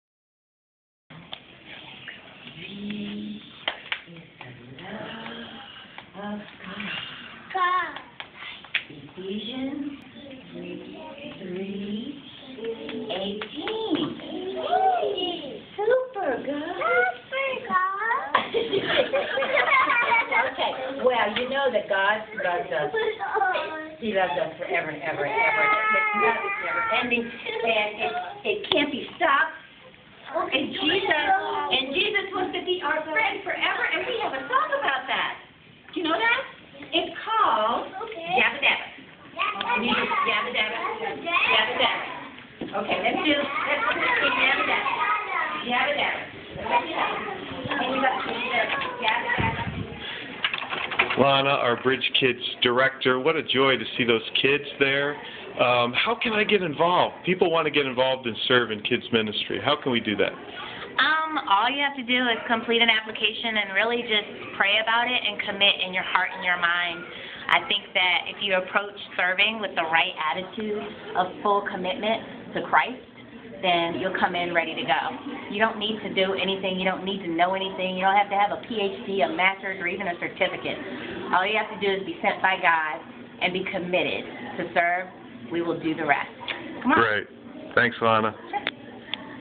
BridgeKids wants YOU!
BK Director Lana invites you to Love Out Loud by serving our precious children - showing them Jesus.